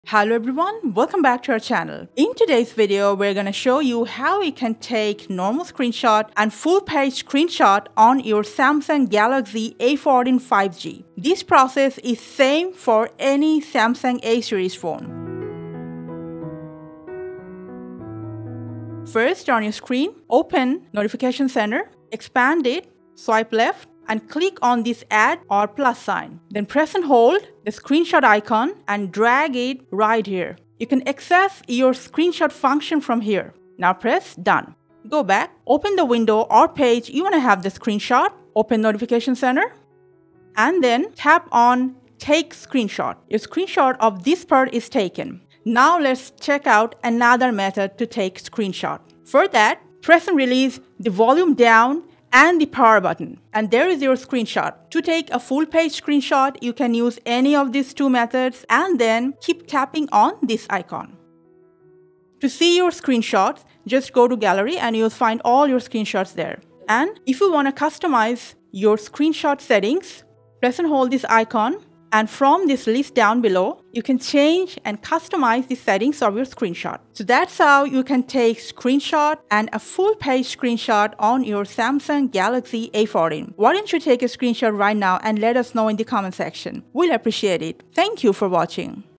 Samsung A14 5G: How to Take Screenshot on Galaxy A Series! [Long/Scrolling]
Looking for a way to take screenshots on a Samsung Galaxy A Series phone with or without using the buttons? Do you have to capture multiple screenshots if you want to save the photo of a complete webpage or long conversation, do you want to capture the whole thing at once?

Here are 2 simple methods to capture screenshots without or with the phone’s Button and also how to take a long or scrolling screenshot on Samsung Galaxy A14 5G, A54 5G, A34 5G, A21s, A31, A41, A51, A71, or any other Galaxy A series Phones effortlessly.

0:00 Samsung Galaxy A14 Screenshots
0:19 Method 1: Screenshot without Button
0:47 Method 2: Screenshot with Button
0:56 Take a Full-screen screenshot
1:07 Customize Screenshot Settings